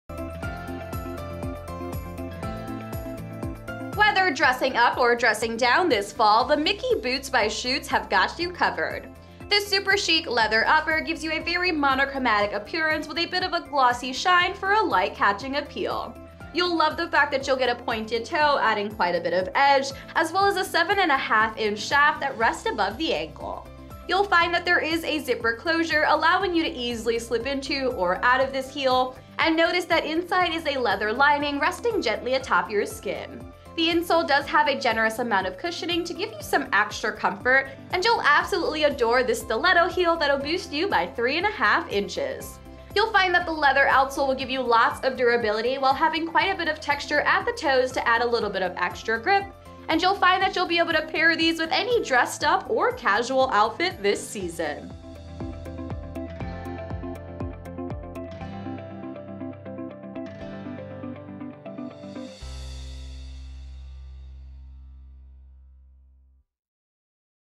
Schutz Mikki SKU: 9728919
Look and feel gorgeous wearing the Schutz&reg; Mikki Boots.
Leather upper.
Leather lining and insole.
Side zippered closure.
Pointed toe silhouette.
Stiletto heel style.
Leather outsole.
Made in Brazil.